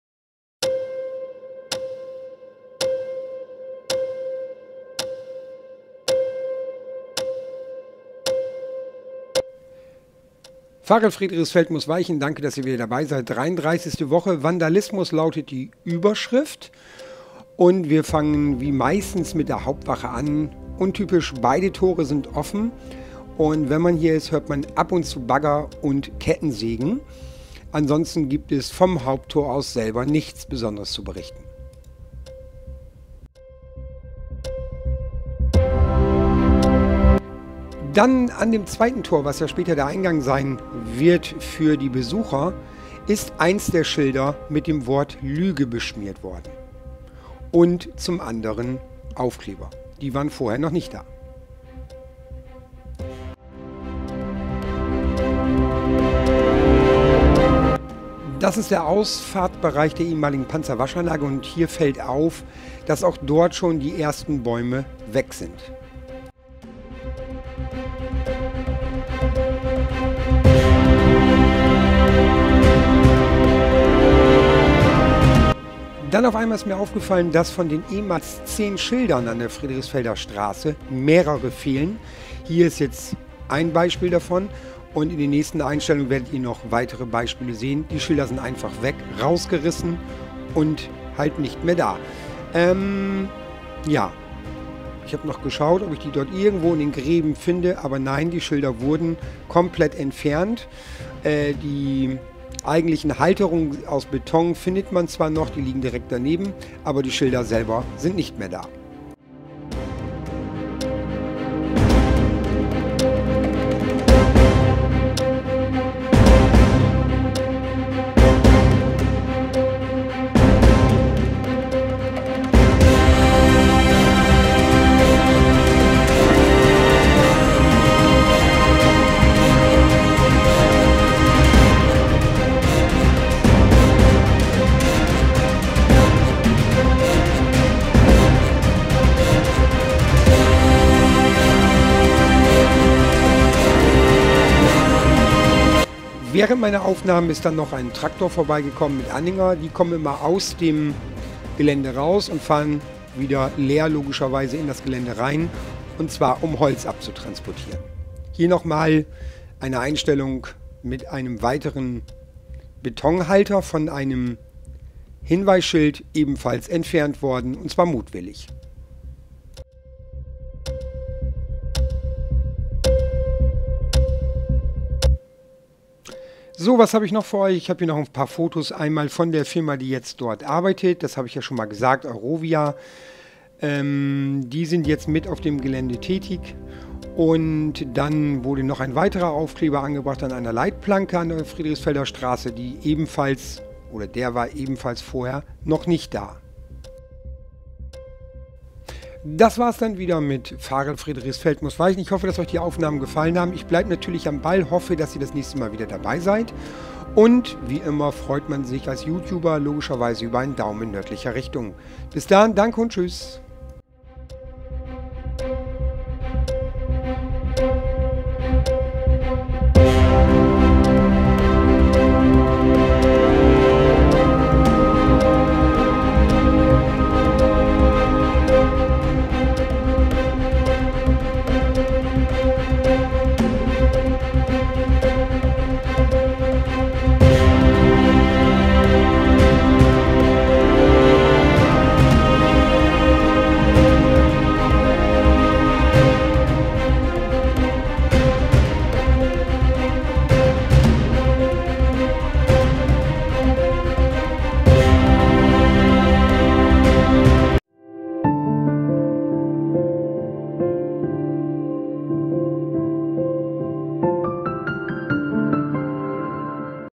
Varel Friedrichsfeld muß weichen 33.Woche Vers.1.3
Der erste Vandalismus auf dem Gelände von dem ehm.Einsatzhafen Varel-Friedrichsfeld.Hier die Bilder.

Vers.1.1:Fehler in Videobeschreibung beseitigt./Videobeschreibung ergänzt u.Suchbegriffe hinzugefügt.
Vers.1.2:???
Vers.1.3:Fehler i.Videoüberschrift beseitigt u.Suchbegriffe ergänzt.

Vers.1.2:Suchbegriffe ergänzt.